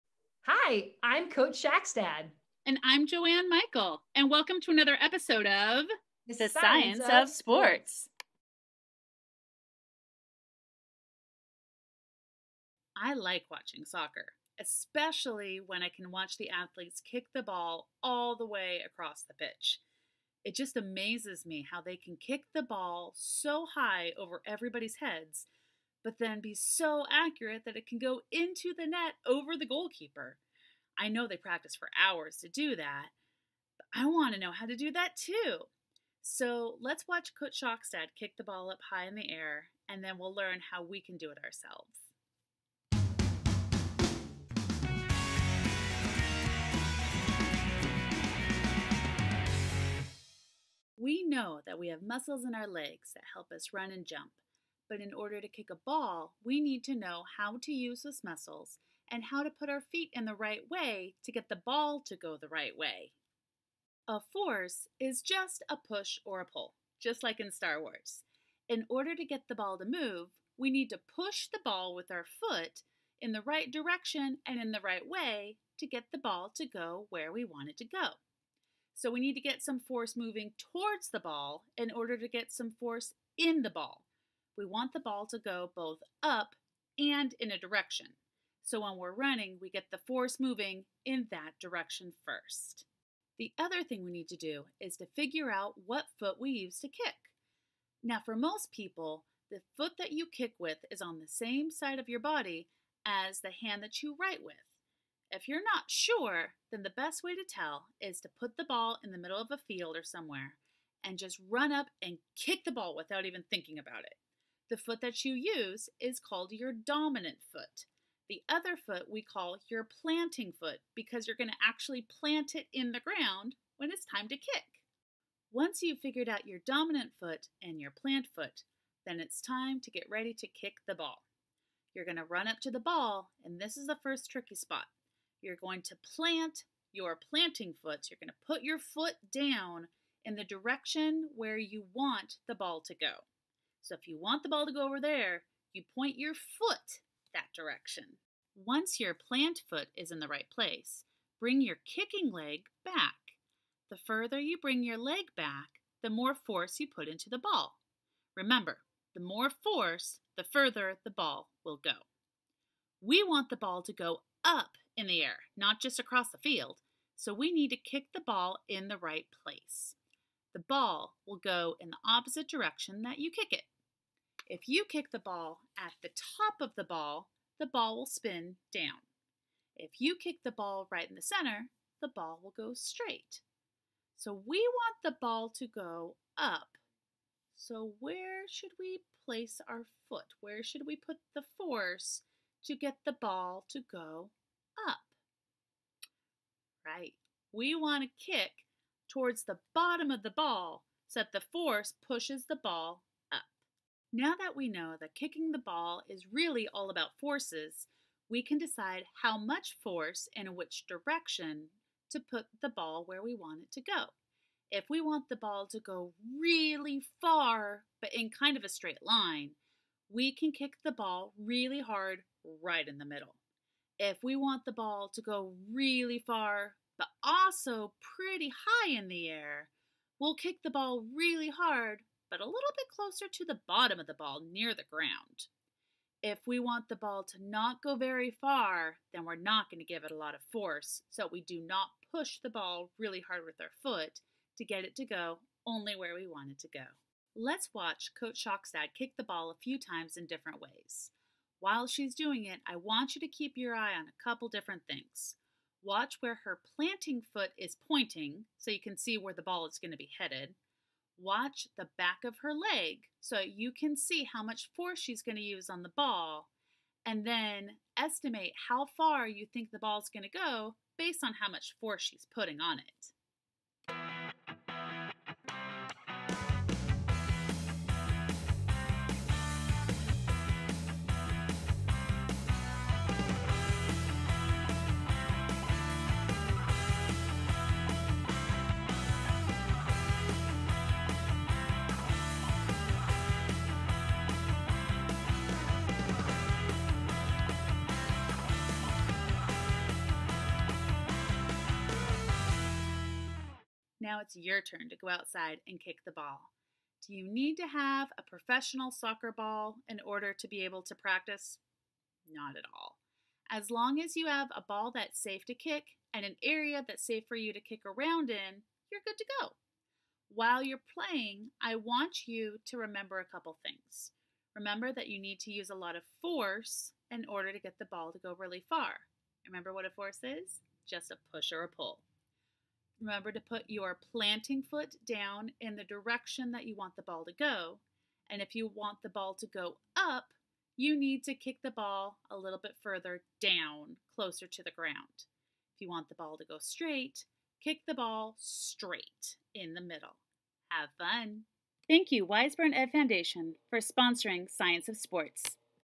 Science of Sports: Kicking a Ball Up and Out
How do we kick a ball up in the air, while still going out a far distance? Watch Coach Shakstad and Mrs. Michael combine forces to make a soccer ball sail through the air! Part of Wiseburn Unified's "Science of Sports" day- Feb 12, 2021.